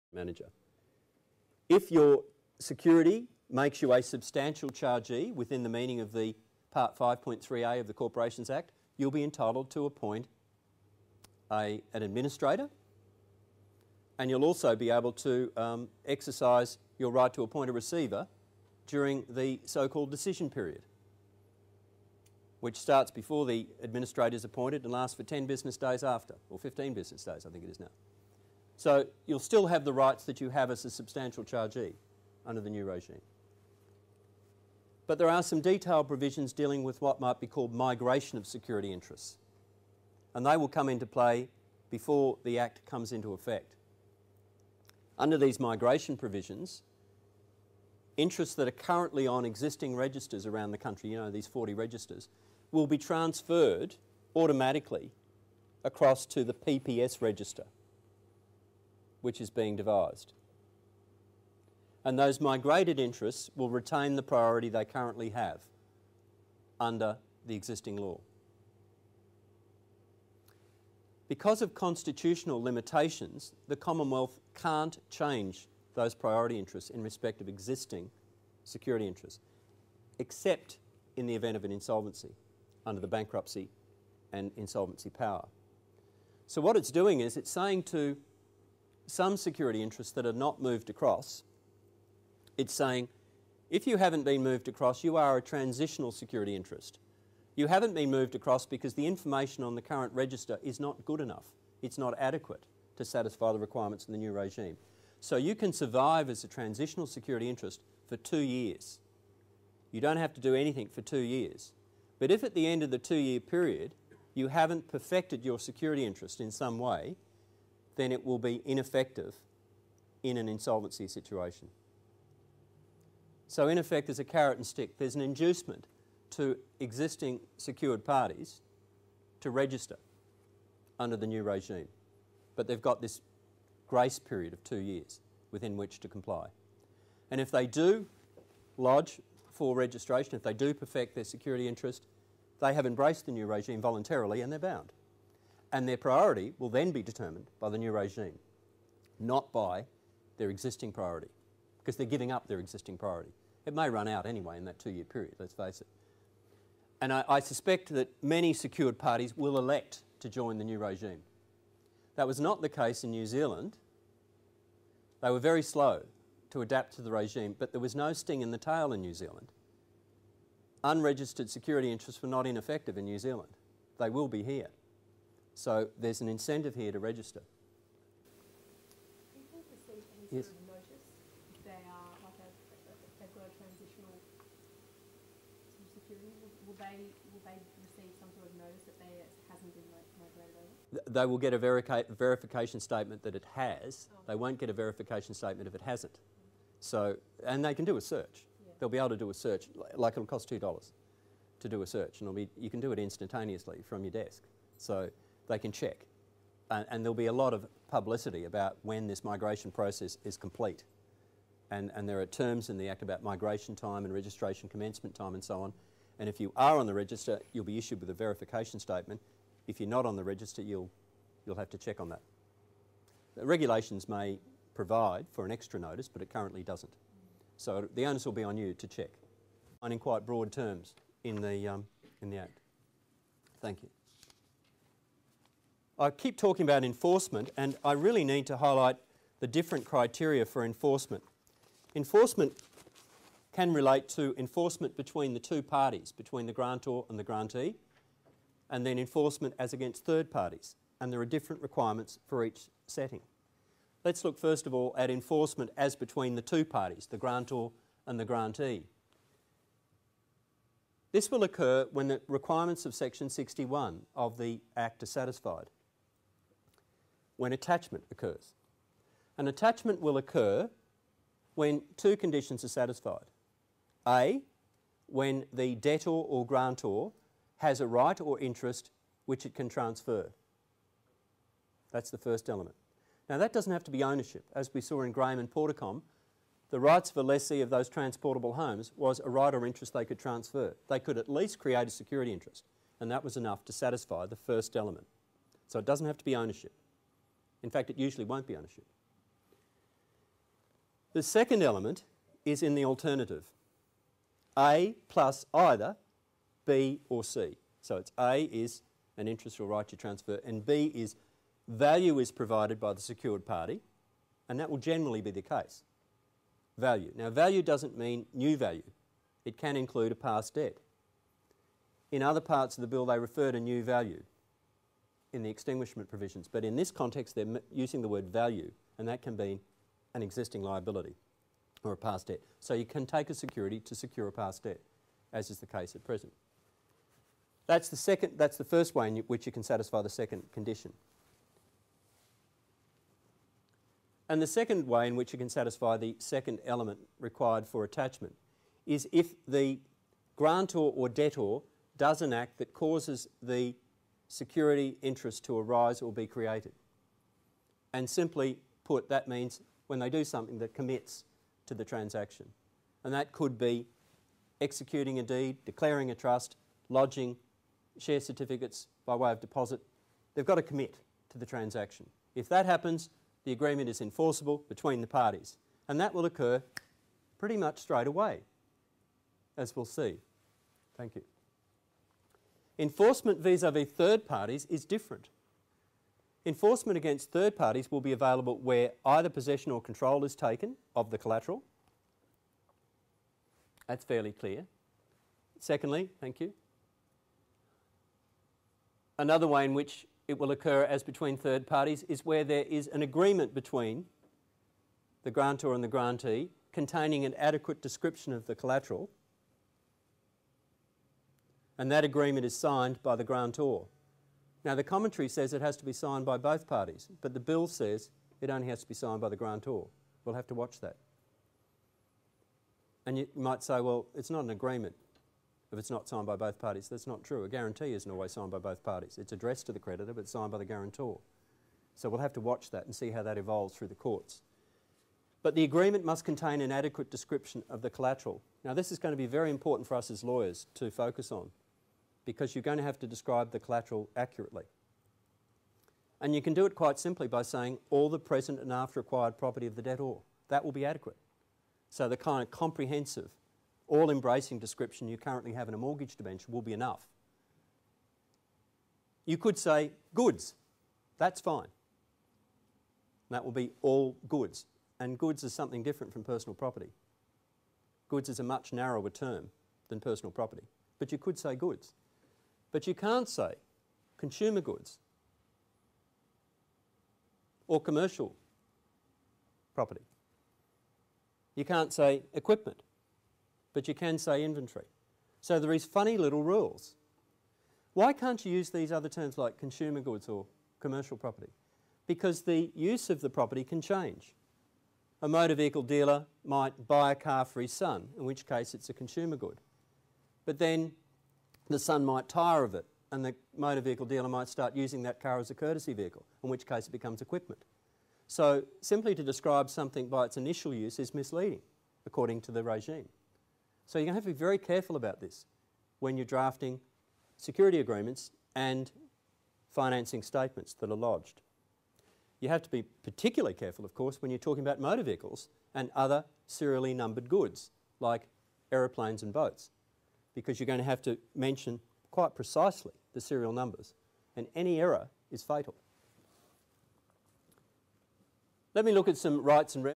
Personal Property Securities Law in Australia 4/8
Author James O'Donovan delivers a presentation on Personal Property Securities reforms in Australia. Over 70 pieces of State and Commonwealth legislation and business practice are involved.  for more information about PPS law reforms in Australia.